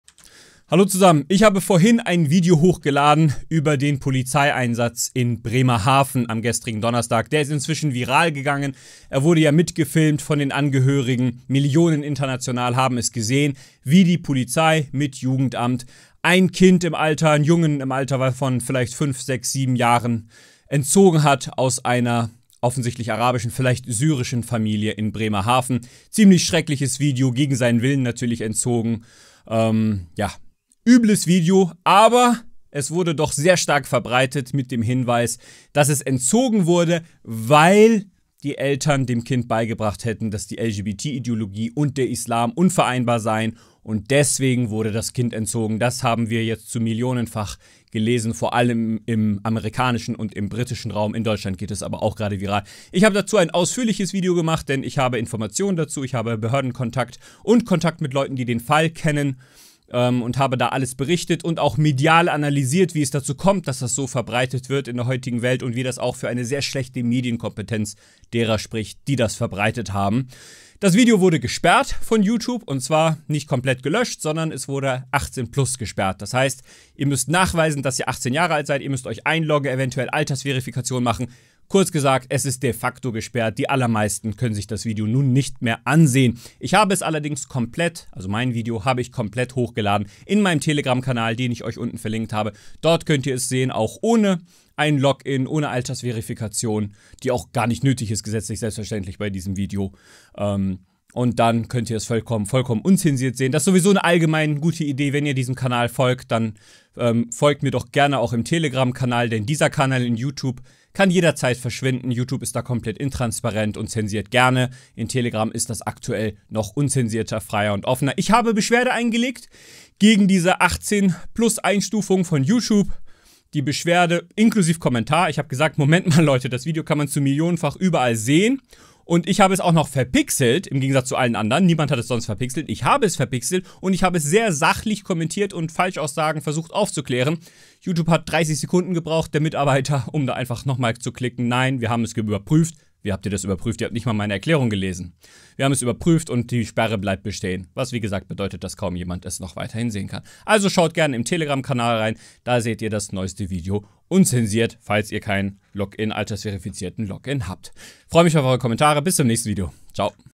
Kindesentzug eines Jungen aus muslimischer Familie | Nachtrag wegen Youtube-Einschränkung des Videos
Polizei in Bremerhaven entzieht muslimischer Familie ihren Sohn, vor ihre Augen, während das Kind sich nach Kräften wehrt. Ein Kommentar.

► Wenn ihr Actuarium fördern wollt, freue ich mich über jede Unterstützung.
► Kein Video mehr verpassen mit dem Telegram-Kanal von Actuarium (inkl. Diskussionsrunde)